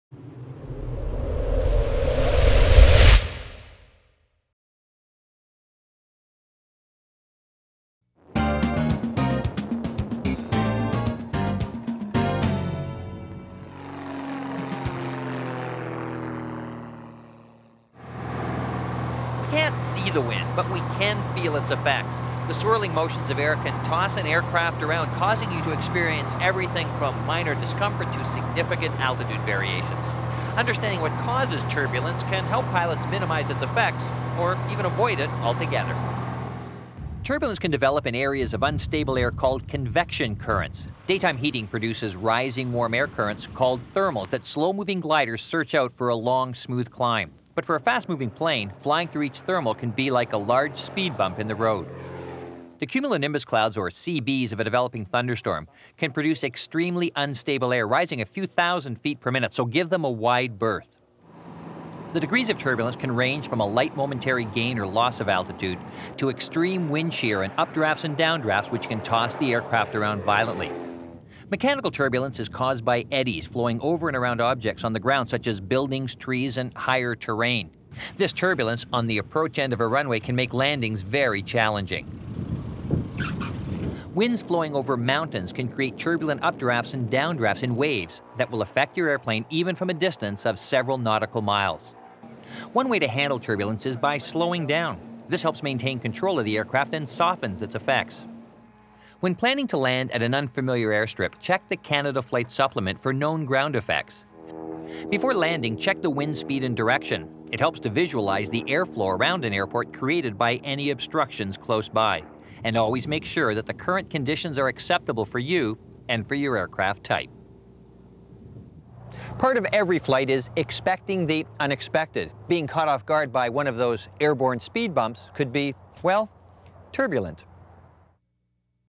SmartPilot | Turbulence
Understanding turbulence and how to handle this condition.